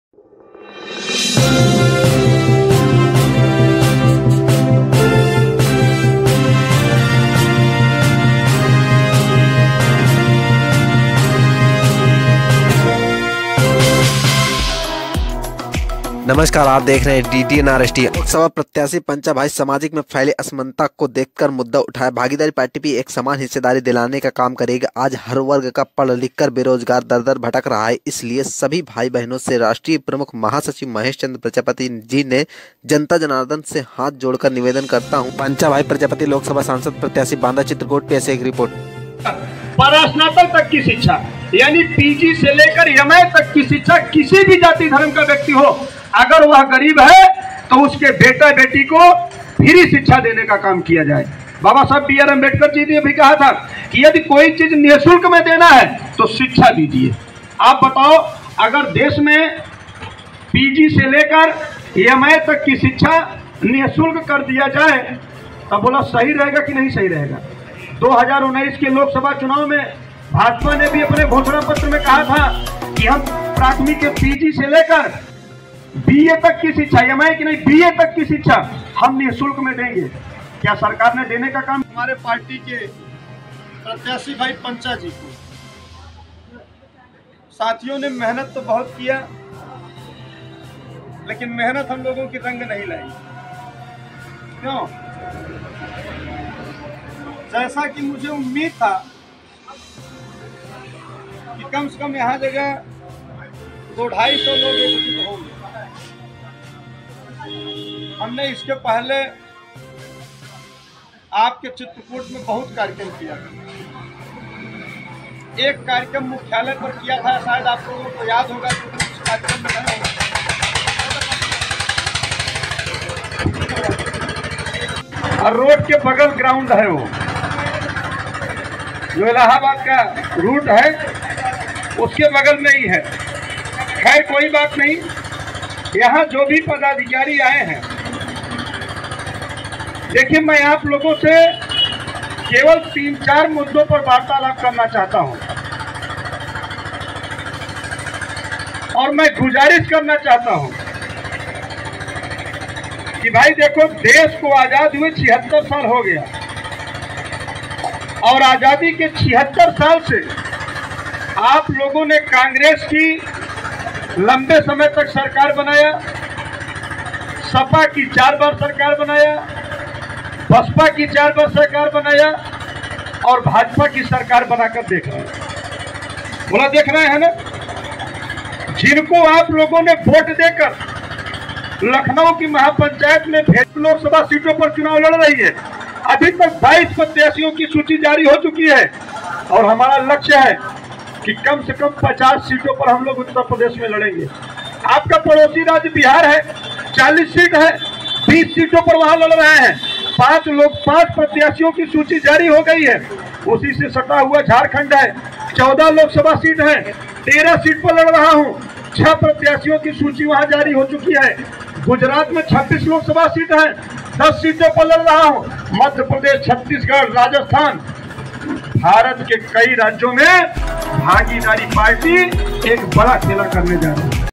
Lok sabha elections: Mahesh Chandra ne लोगो से निवेदन करते हुए जनता को जगरूप किया | DDNRST
2024 lok sabha election
2024 lok sabha elections